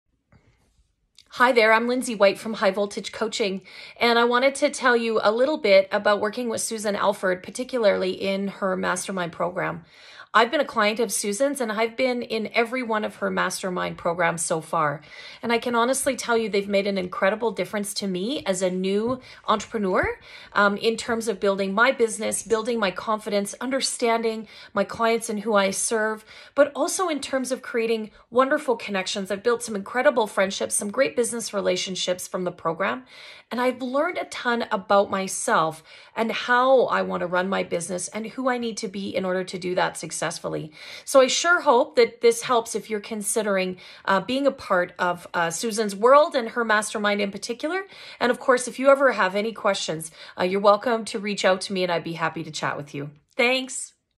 Client Testimonial: Lindsay White
Here's what Lindsay has to say about her experience in the Accelerate! Mastermind.